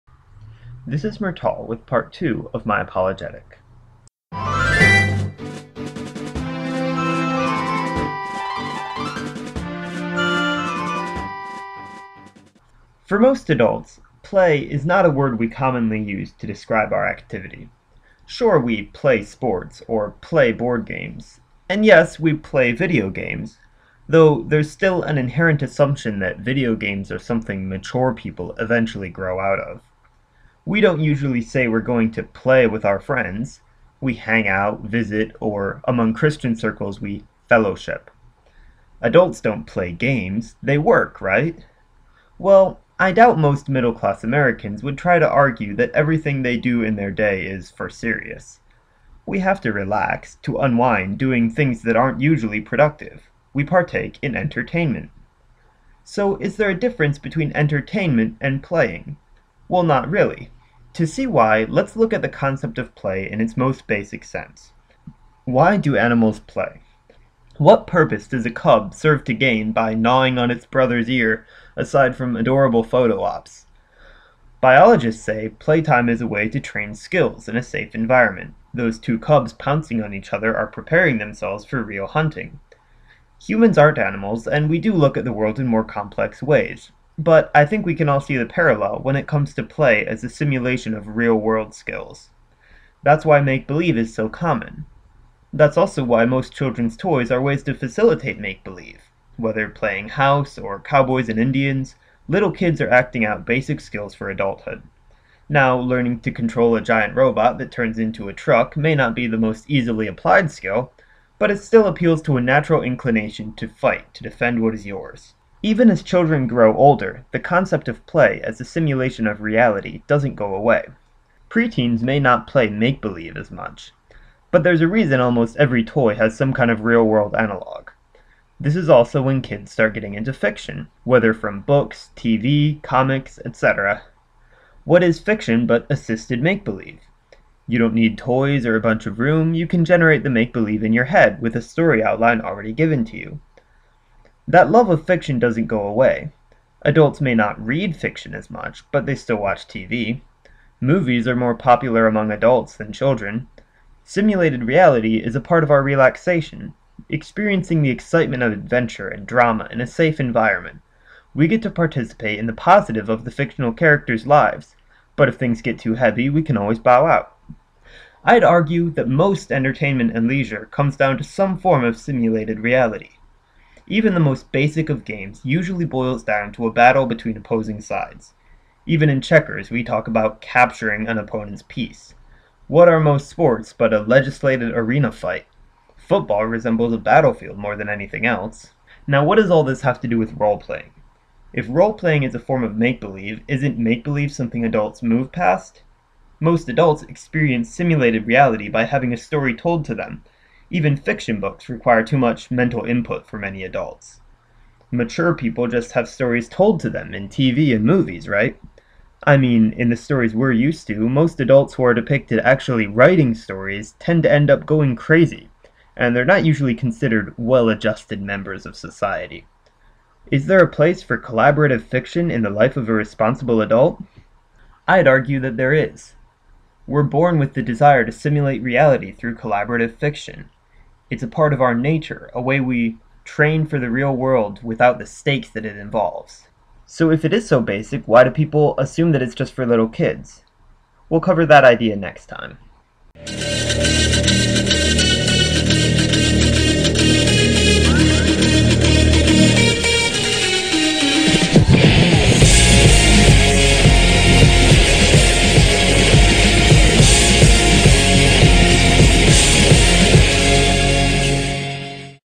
An RPG Apologetic, Part Two: The Concept of Play
I take a look at what defines recreation, and how collaborative storytelling is not only normal, but inherent in how we define relaxation.